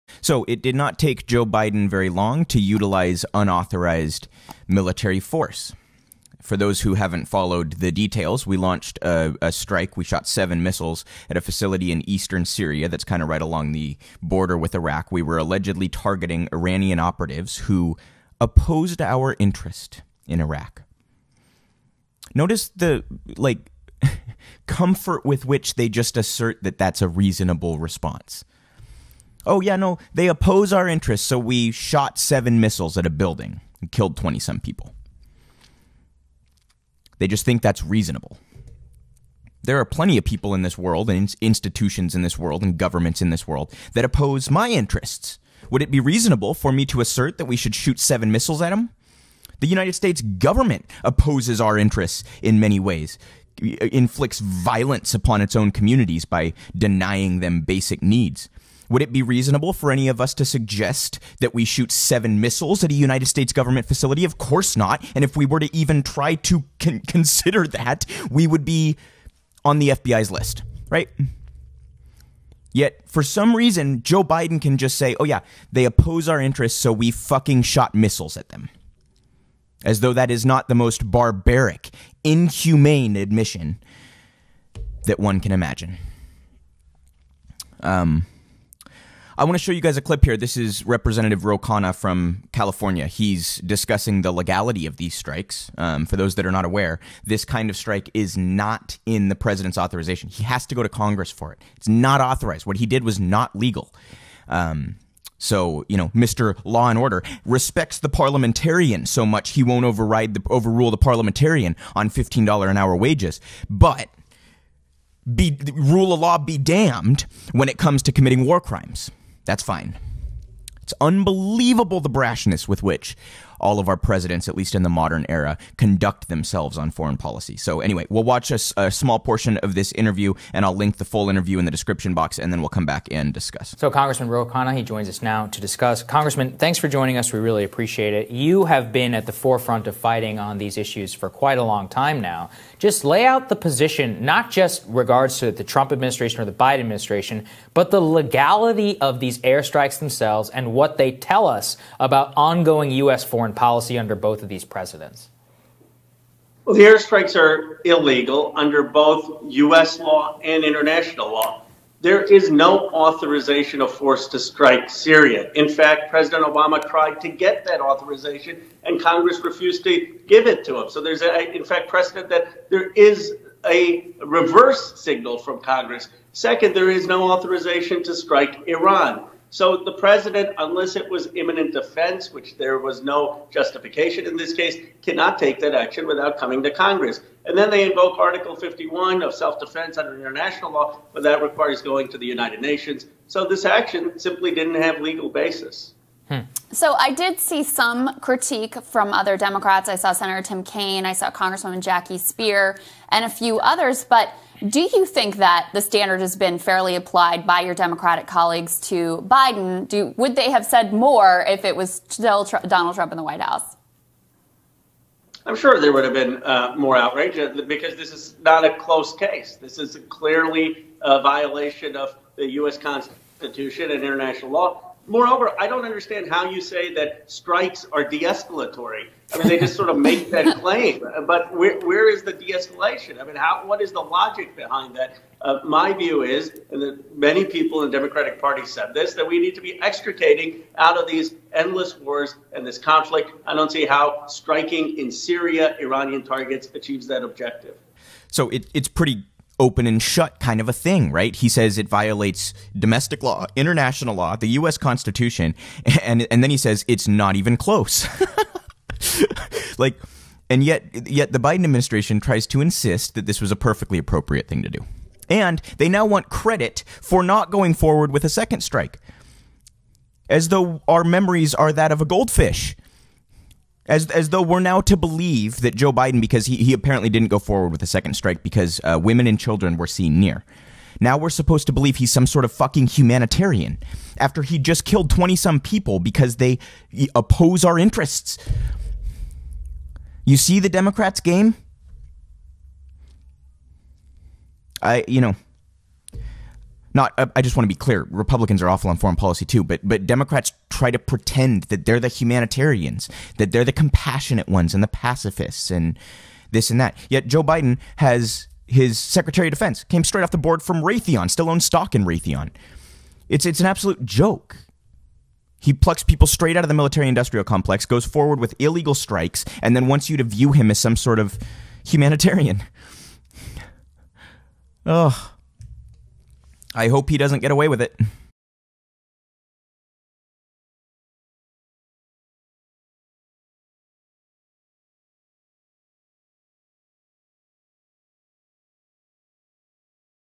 Biden's Unauthorized Strike In Syria
Joe Biden launched an unauthorized, illegal strike in Syria, killing at least 22 people.